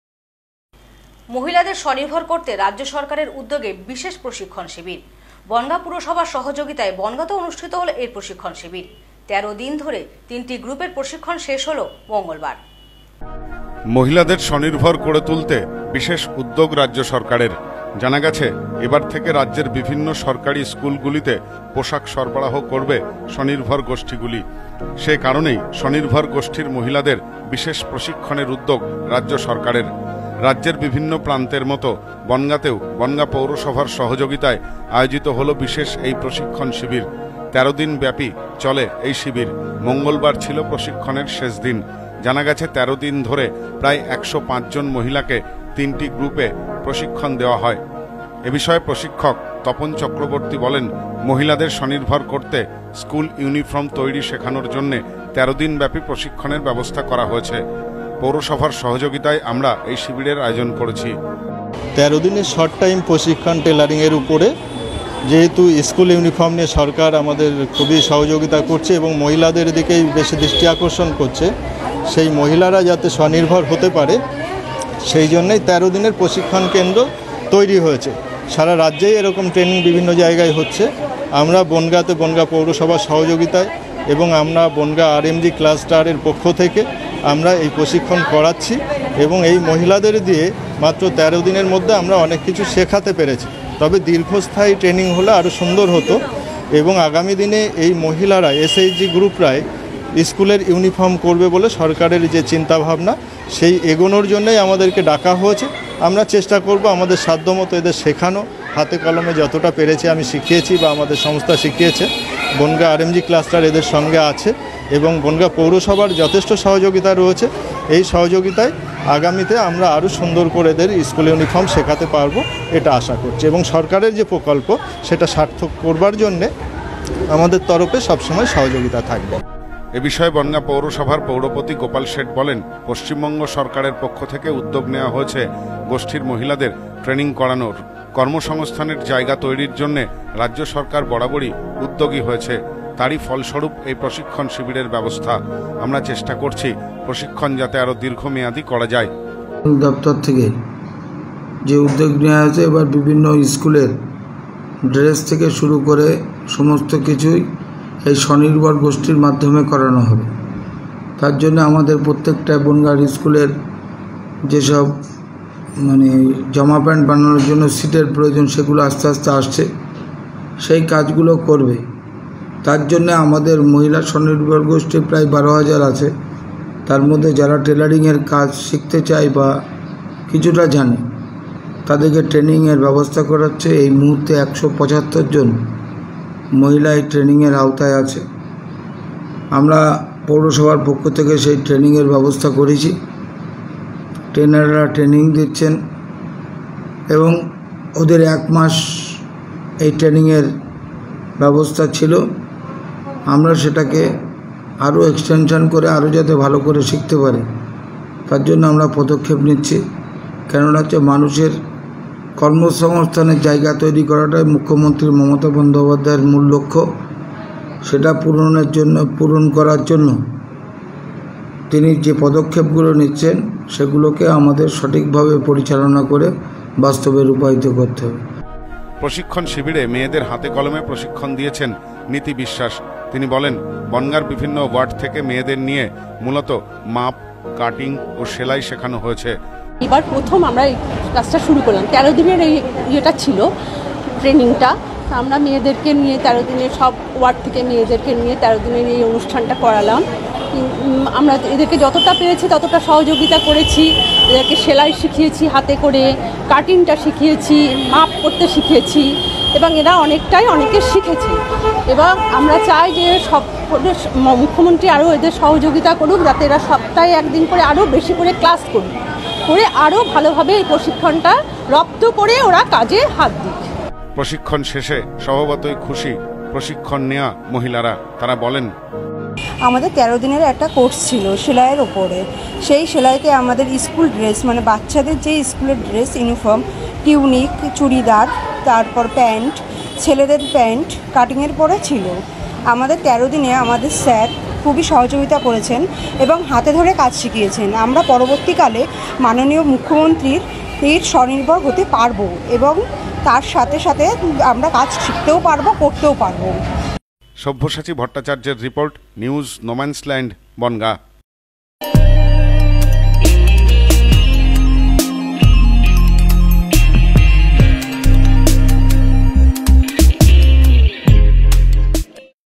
মহিলাদের স্বনির্ভর করতে বিশেষ প্রশিক্ষণ শিবির বনগাঁয় ৷
মহিলাদের স্বনির্ভর করতে রাজ্য সরকারের উদ্যোগে বিশেষ প্রশিক্ষণ শিবির। বনগাঁ পৌরসভার সহযোগিতায় বনগাঁতেও অনুষ্ঠিত হল প্রশিক্ষণ শিবির। ১৩ দিন ধরে ৩ টি গ্রুপের প্রশিক্ষণ শেষ হয় মঙ্গলবার।